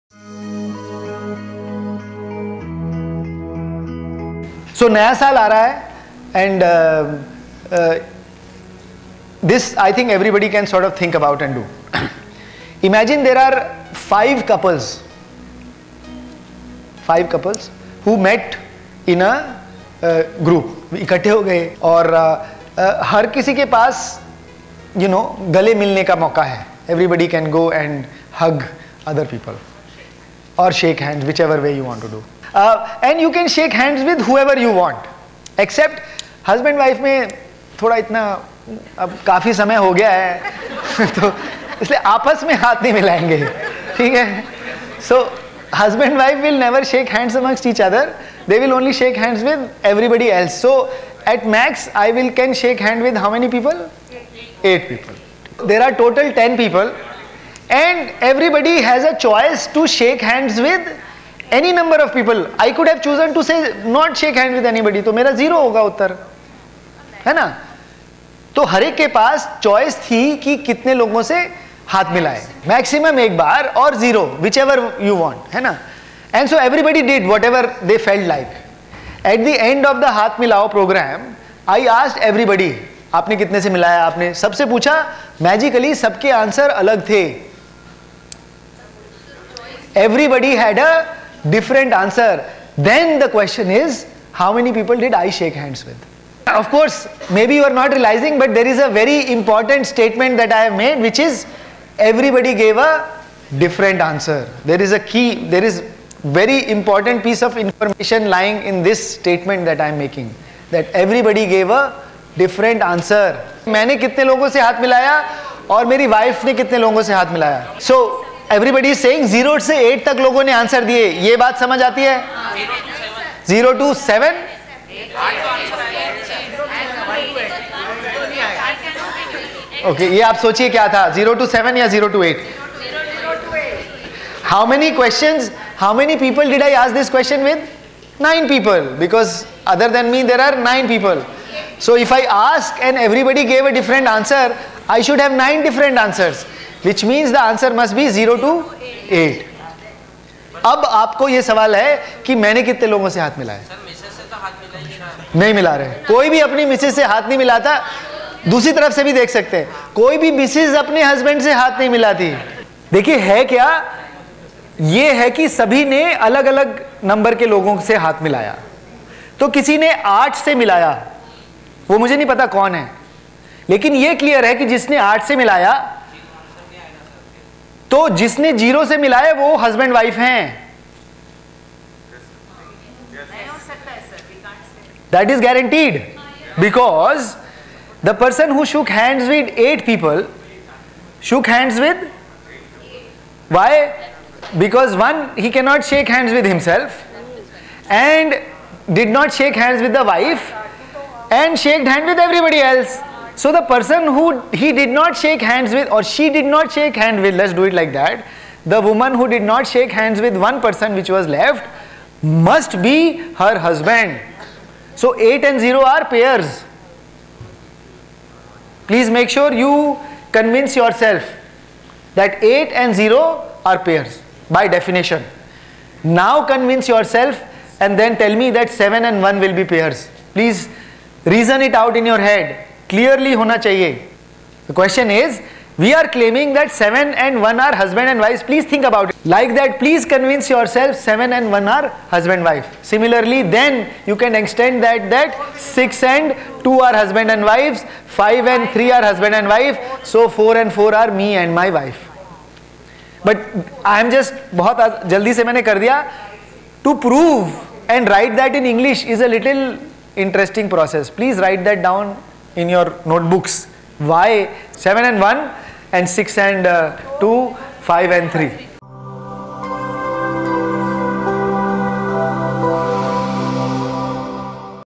New Year Hug | Math Puzzle
Imagine there are 5 couples, and everyone can shake hand/hug each other except his/her own husband or wife. It is not necessary that everyone shakes hand with every other. They can shake hand with number of people as they wish. So, the number of people they can shake hand maximum becomes 8 and minimum becomes zero. At the end of the program, we asked we each one how many people did each shake hand with. Interestingly, everyone's answer was different.

Now, the question is How Many People did I Shake Hands/Hug With?
About Us